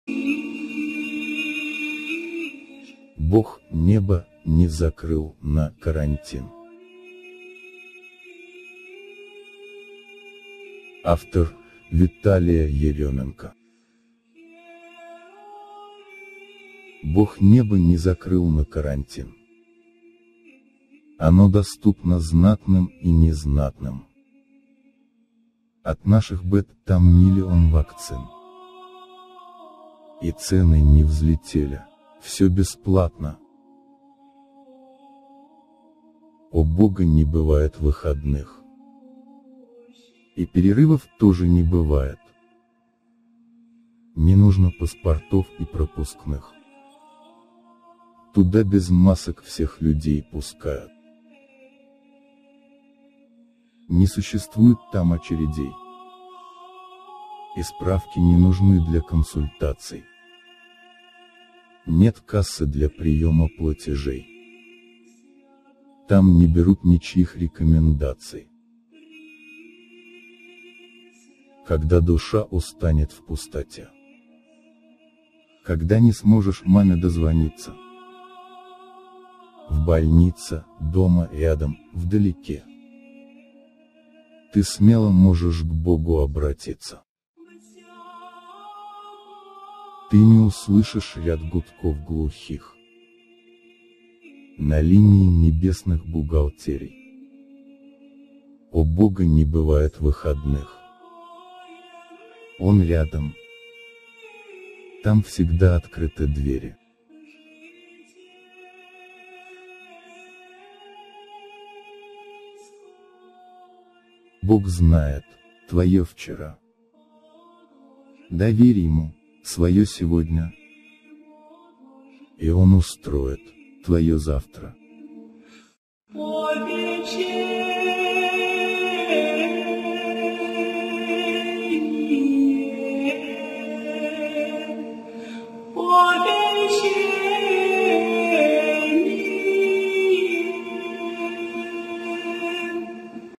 Бог небо не закрыл на карантин. God did not quarantine the sky.
БОГ НЕБО НЕ ЗАКРЫЛ НА КАРАНТИН
Автор - Виталия Ерёменко

Оно доступно знатным и незнатным.
От наших бед там миллион вакцин.
И цены не взлетели. Всё бесплатно.

У Бога не бывает выходных,
И перерывов тоже не бывает.
Не нужно паспортов и пропускных,
Туда без масок всех людей пускают.

Не существует там очередей,
И справки не нужны для консультаций.
Нет кассы для приёма платежей,
Там не берут ничьих рекомендаций.

Когда душа устанет в пустоте,
Когда не сможешь маме дозвониться.
В больнице, дома, рядом, вдалеке,
Ты смело можешь к Богу обратиться.

Ты не услышишь ряд гудков глухих
На линии небесных бухгалтерий.
Он рядом. Там всегда открыты двери.
GOD HAS NOT COVERED THE SKY ON QUARANTINE
Author - Vitalia Eremenko

God did not quarantine the sky,
It is available to the noble and the ordinary.
There are a million vaccines for our troubles.
And prices didn't skyrocket. Everything is free.

God never has days off
And there are no breaks either.
No need for passports and passes,
All people are allowed there without masks.

There are no queues there
And no information is needed for consultation.
There is no cash desk for accepting payments,
They don't take anyone's recommendations.

When the soul gets tired in emptiness
When you can't get through to mom.
In the hospital, at home, nearby, in the distance,
You can boldly turn to God.

You won't hear a row of deaf beeps
On the line of heavenly bookkeepers.
He is near. The doors are always open there.